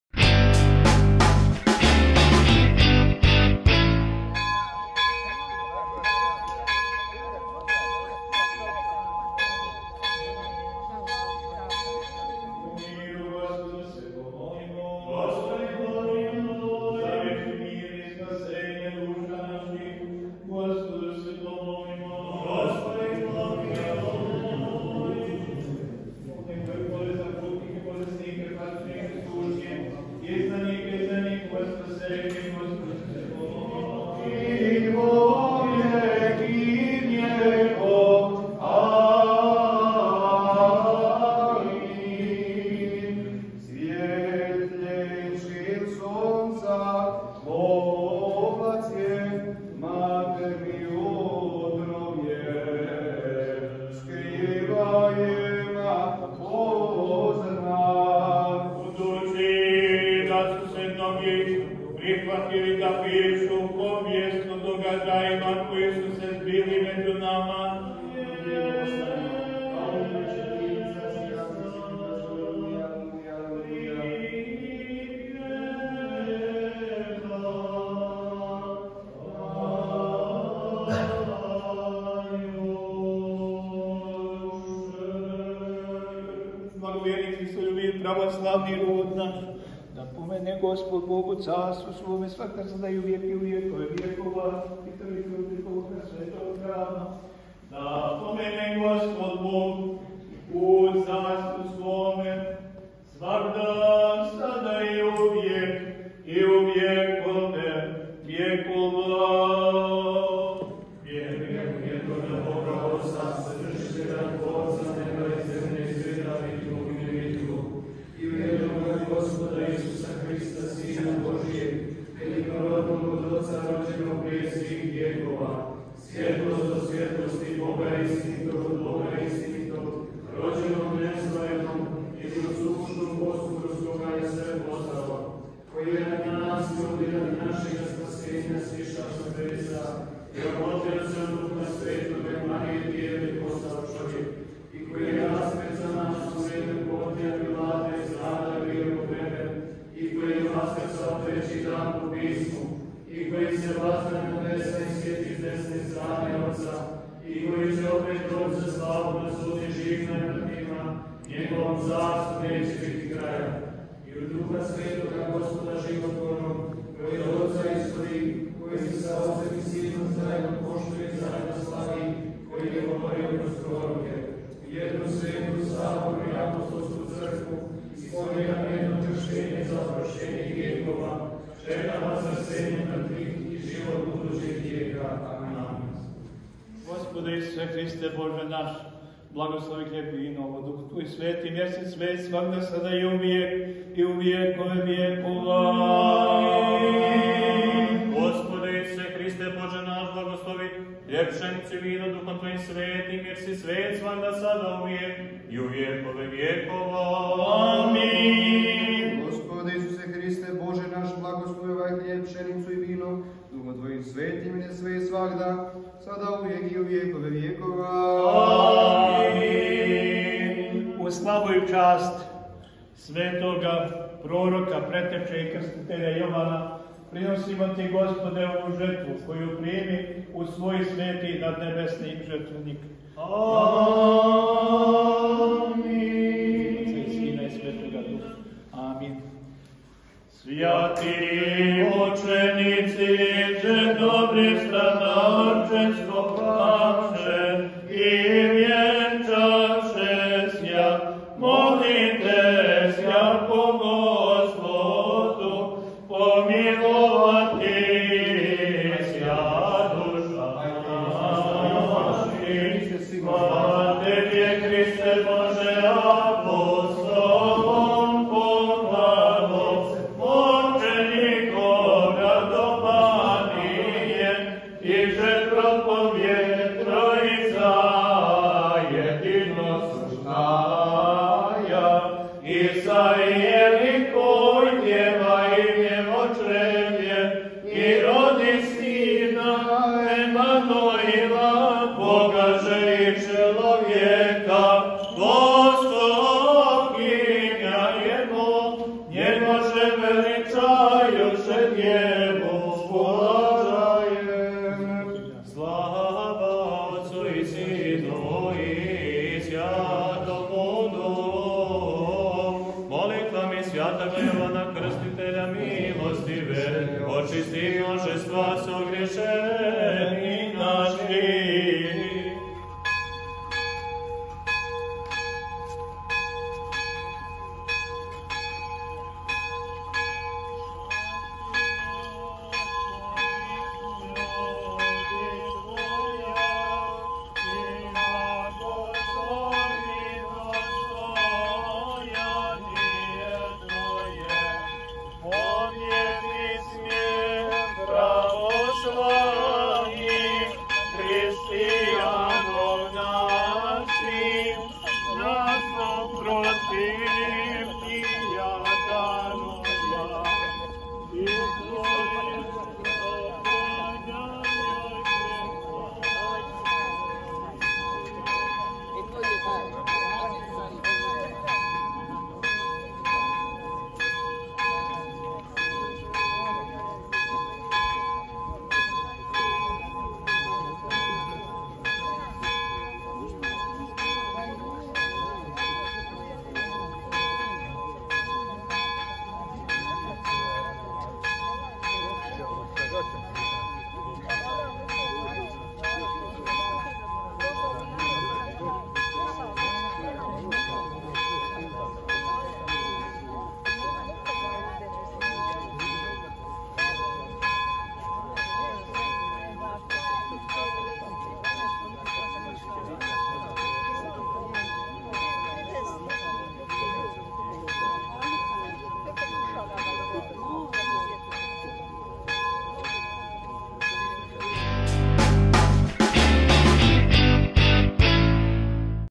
Obilježena slava Crkve Začeća sv. Jovana Krstitelja na Golom Brdu
U petak, 6. oktobra 2023. godine na Golom Brdu obolježena je slava pravoslavne crkve. Kuma slave bila je Danica Perić, dok se za narednu godinu prijavila Rajka Milanović, sa trenutnom adresom stanovanja u Australiji.